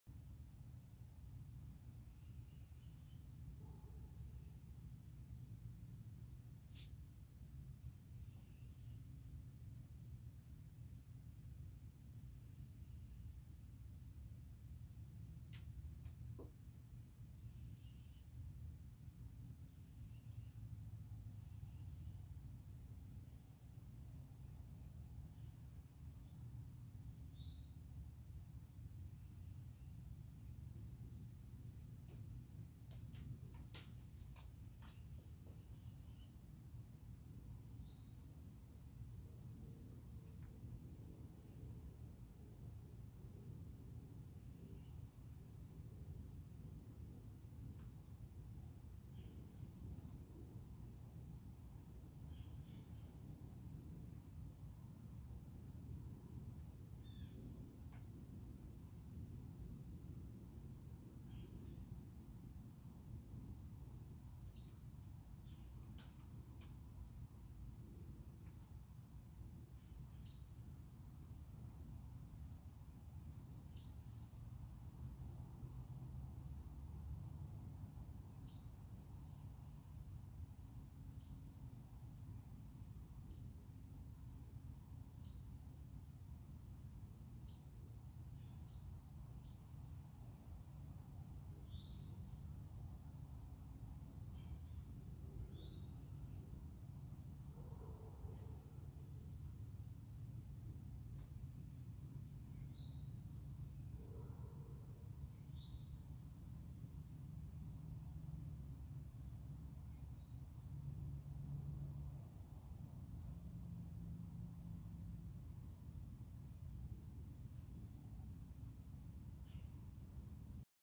Two-Minute Moment: Calm Natural Forest
A spot of natural forest video.
Some animal sounds.
It's Sunday morning so the mowers are not out yet.

Enjoy the peace for your day!